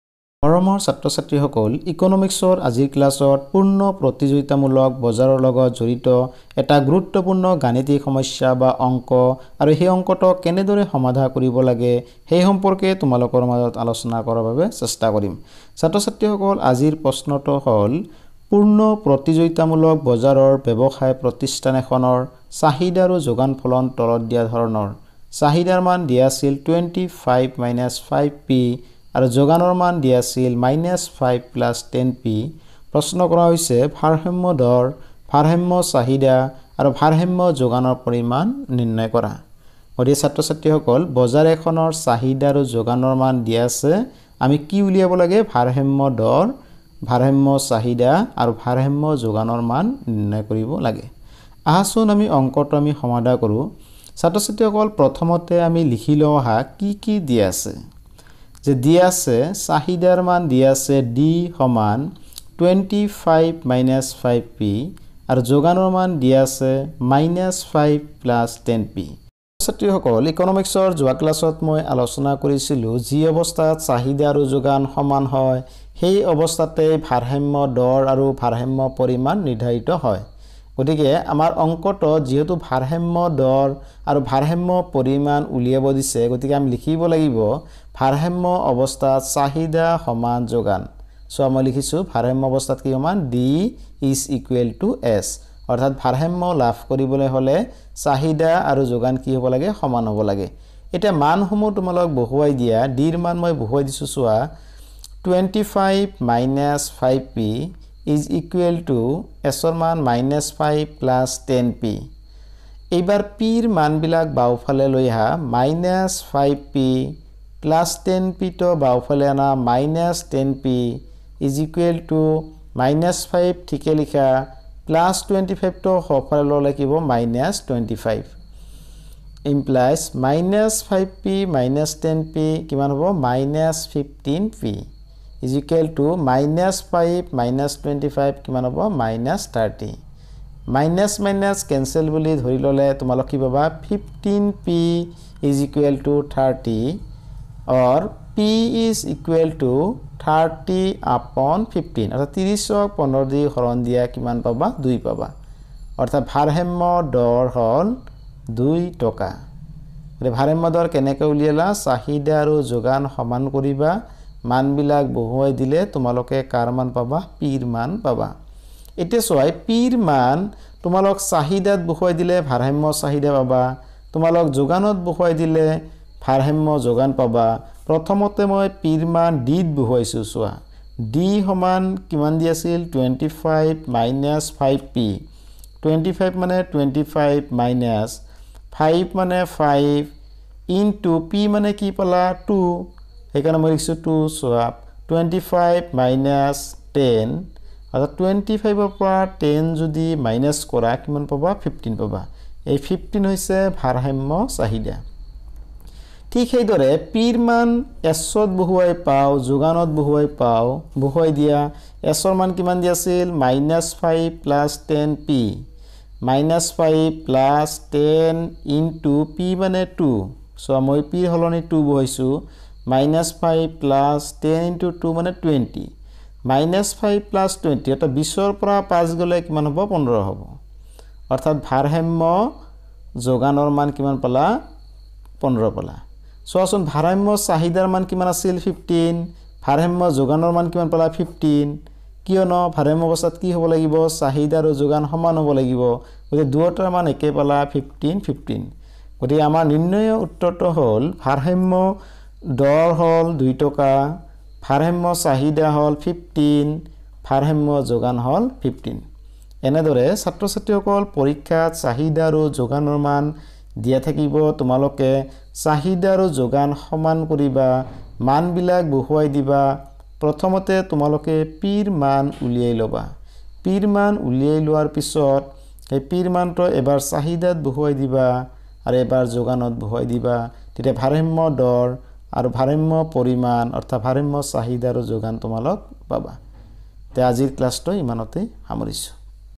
বজাৰ : পূৰ্ণ প্ৰতিযোগিতামূলক বজাৰ - গাণিতিক সমস্যা আৰু সমাধান
Market : Perfect competition market, determination of equilibrium price and output